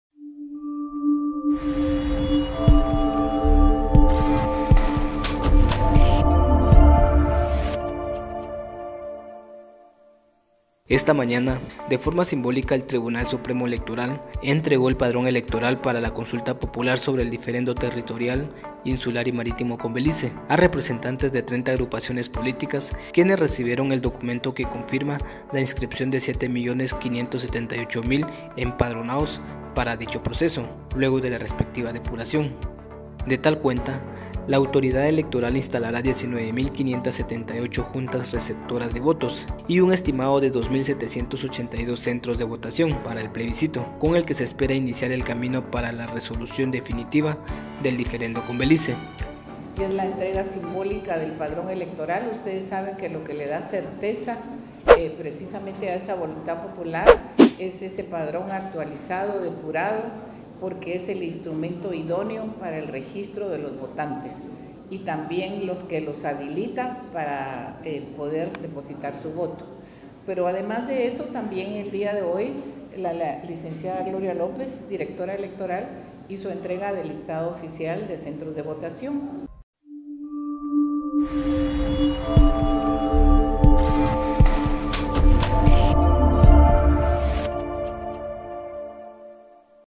TSE confirma que la consulta popular convocará a 7.5 millones de ciudadanos
Esta mañana, de forma simbólica, el Tribunal Supremo Electoral (TSE) entregó el padrón electoral para la consulta popular sobre el diferendo territorial, insular y marítimo con Belice, a representantes de 30 agrupaciones políticas, quienes recibieron el documento que confirma la inscripción de 7 millones 578 mil empadronados para dicho proceso, luego de la respectiva depuración.

De tal cuenta, la autoridad electoral instalará 19 mil 578 juntas receptoras de votos y un estimado de 2 mil 782 centros de votación para el plebiscito, con el que se espera iniciar el camino para una resolución definitiva del diferendo con Belice.

Así, cientos de colaboradores trabajan en la preparación de las cajas que incluirán las papeletas, marcadores, lapiceros, cinta adhesiva y tinta indeleble, todos insumos electorales para la efectiva emisión del sufragio. La distribución de los kits para las mesas se iniciará luego de la Semana Santa, aclaró el TSE.

Con información de Sara Solorsano